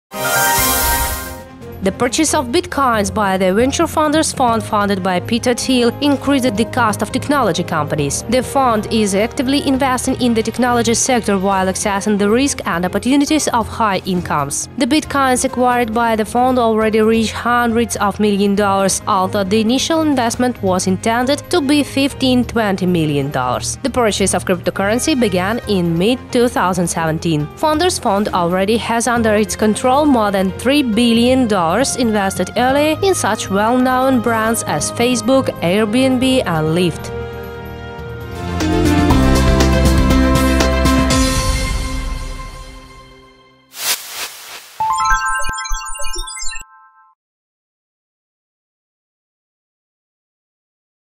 Venture Fund focuses on bitcoin
The purchase of bitcoins by the venture Founders Fund, founded by Peter Thiel, increases the cost of technology companies. The fund is actively investing in the technology sector while assessing the risks and opportunities of high incomes. The fund is actively investing in the technology sector while assessing the risks and opportunities of high incomes. The bitcoins acquired by the fund already reach hundreds of millions of dollars, although the initial investment was intended to be $15-20 million. The purchase of cryptocurrency began in mid-2017. Founders Fund already has under its control more than $ 3 billion invested earlier in such well-known brands as Facebook, Airbnb, and Lyft.